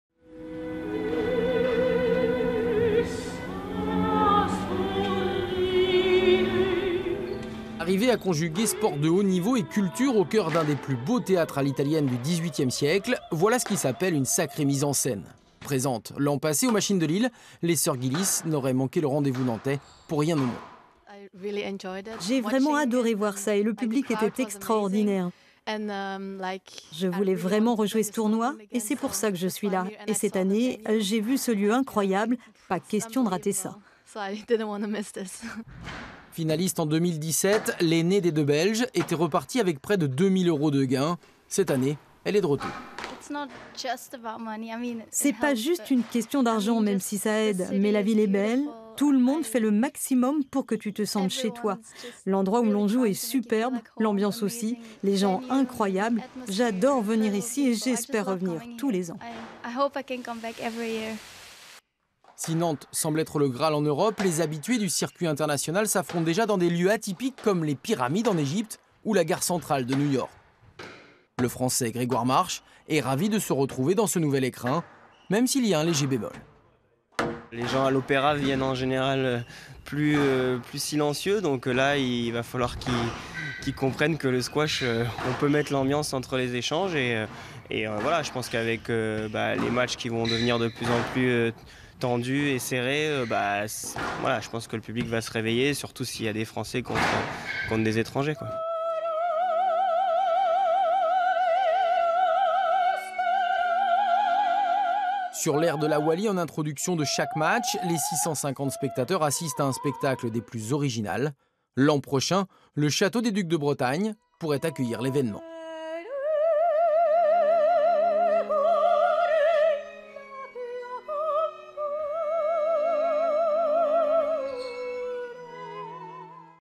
Nantes : du squash au théâtre Graslin
Du sport qui se mélange à la culture. L'Open International de Squash de Nantes s'invite au Théâtre Graslin. 50 joueurs et joueuses venus du monde entier. Anthony Brulez et Frédéric Grunchec ont recueilli leur sentiment sur ce tournoi qui sort de l'ordinaire